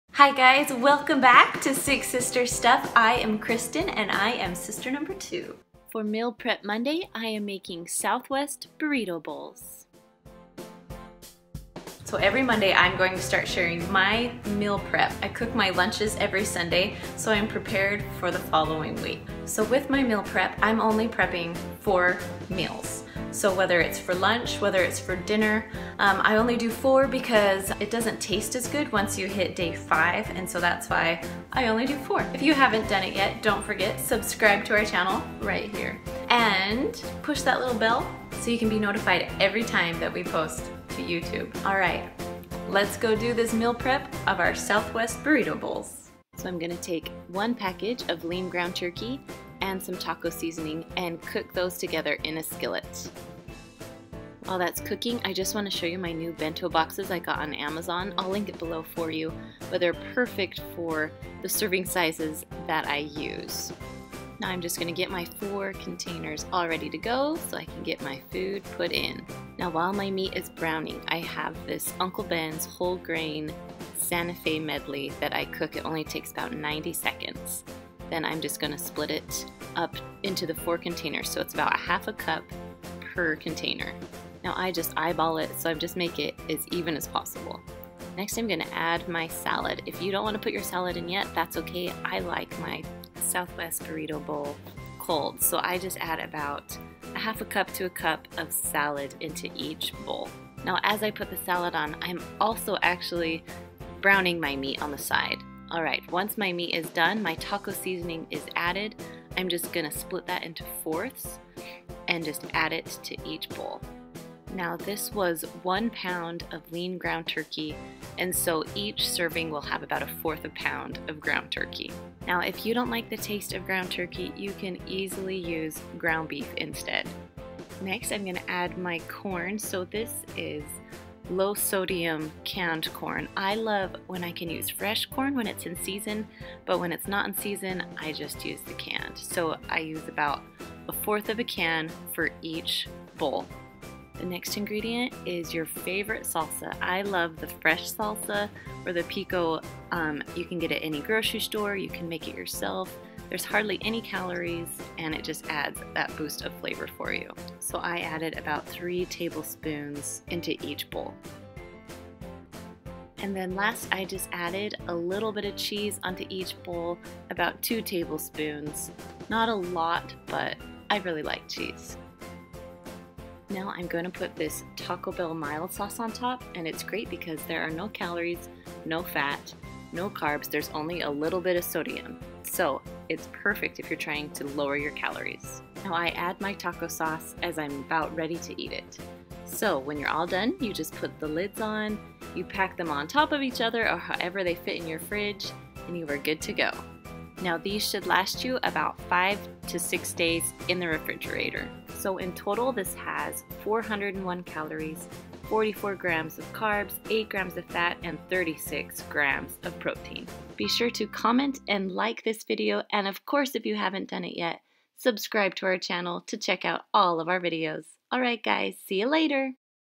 Southwest Burrito Bowls Meal prep Monday (AKA Healthy Taco Salad) Week 2
WELCOME BACK to Meal Prep Monday week 2! With this recipe being only 400 Calories, you can't go wrong with these Southwest Burrito Bowls (also known as HEALTHY TACO SALAD!) From start to finish this meal only takes 30 minutes and you have lunch (or dinner) for 4 days! This Burrito Bowl is full of flavor and full of protein! If you love our meal prep recipes, please comment and like our video!

44 grams of carbs
8 grams of fat
36 grams of protein